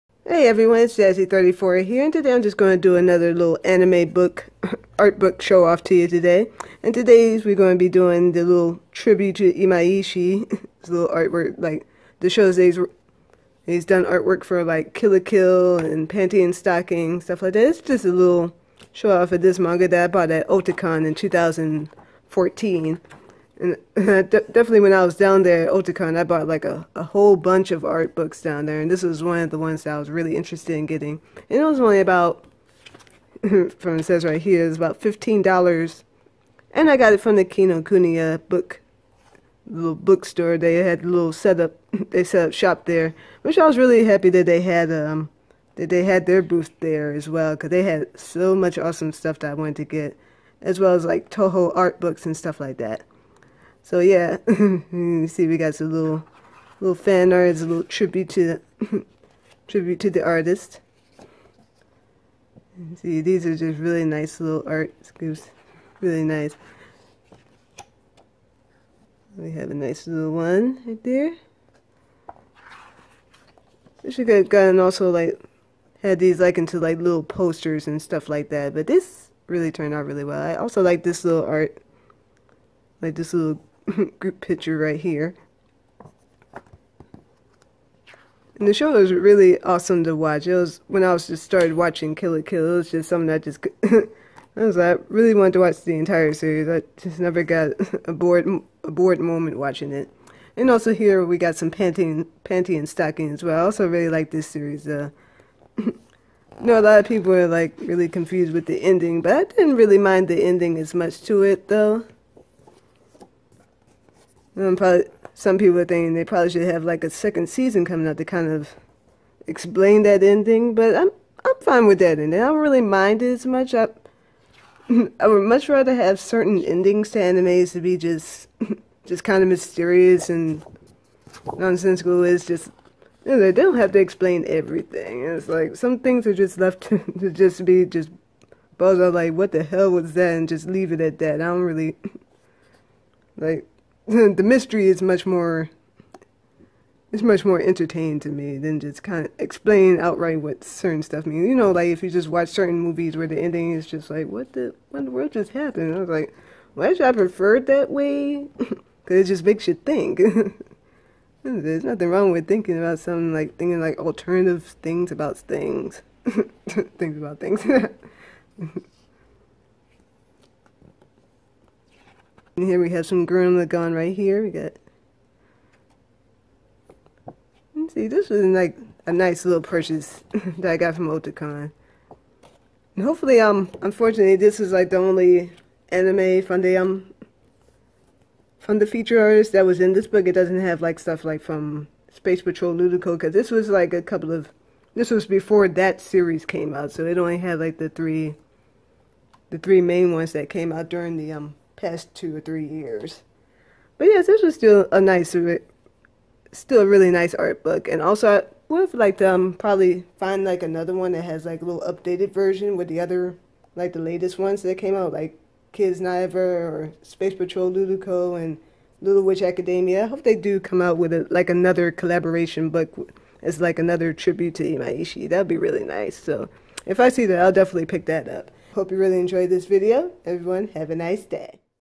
Imaishi Tribute Artbook
Here's another anime art show-off from my collection.  I hope you will enjoy this video.